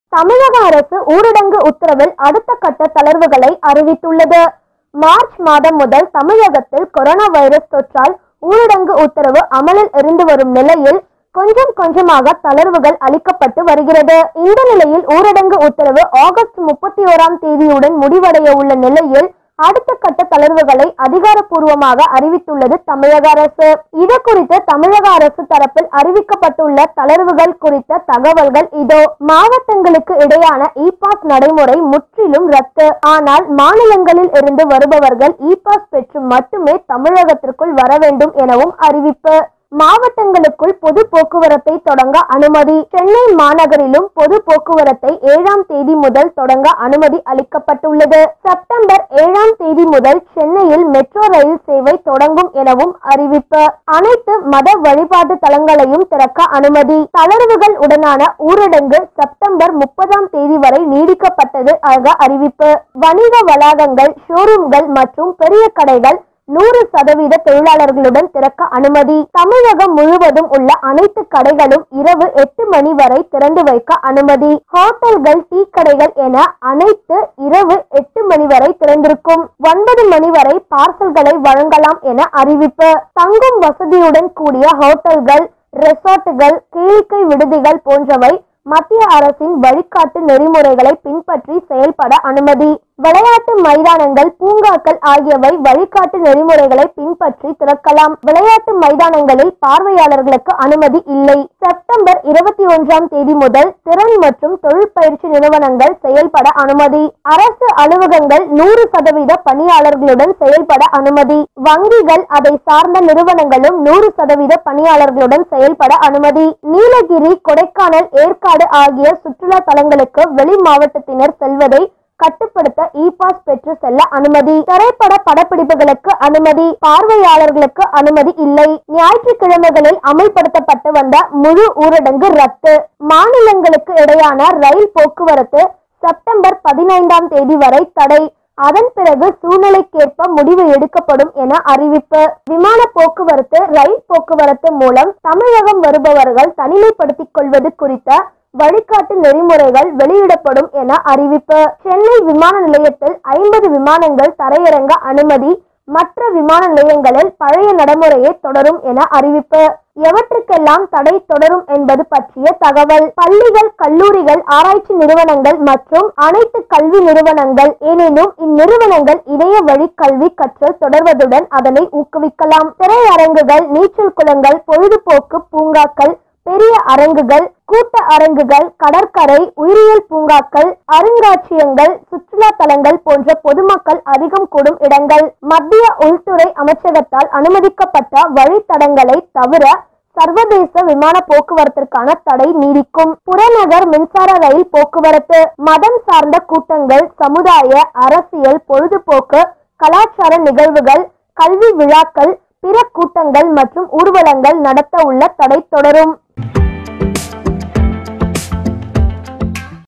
மெட்ரோ ரயில் , பஸ்கள் இயங்க அனுமதி - அடுத்த கட்ட தளர்வுகளை அறிவித்தது தமிழக அரசு.!! | Unlock 4.0
மெட்ரோ ரயில் , பஸ்கள்  இயங்க அனுமதி - அடுத்த கட்ட தளர்வுகளை அறிவித்தது தமிழக அரசு.!!